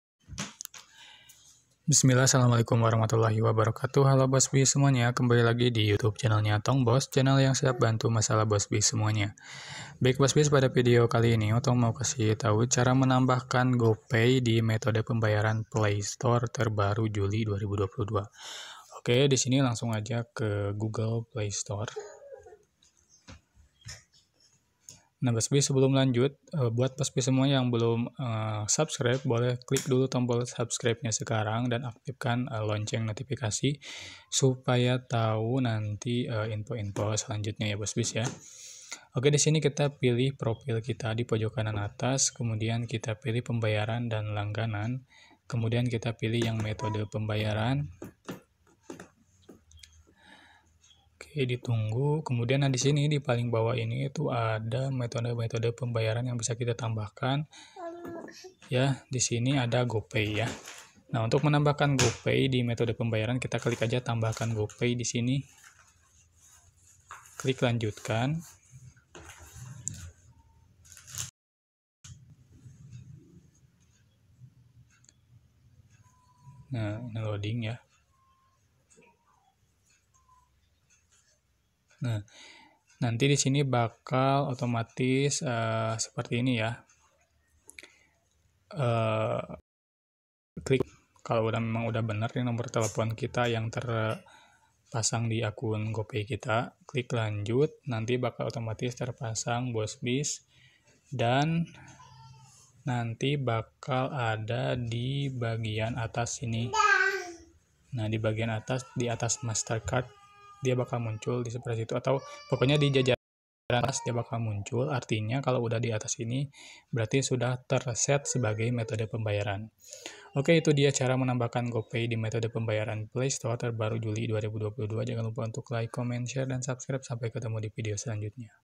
Cara Menambahkan GOPAY di Metode Pembayaran Playstore Terbaru Juli 2022
Hallo bos bis dalam video ini otong bakal sharing ke Bos Bis semua, Cara Menambahkan GOPAY di Metode Pembayaran Playstore Terbaru Juli 2022,   bagi bos bis yang belum tau yuk simak videonya dan jangan di skip ya biar paham, mudah mudahan bermanfaat :)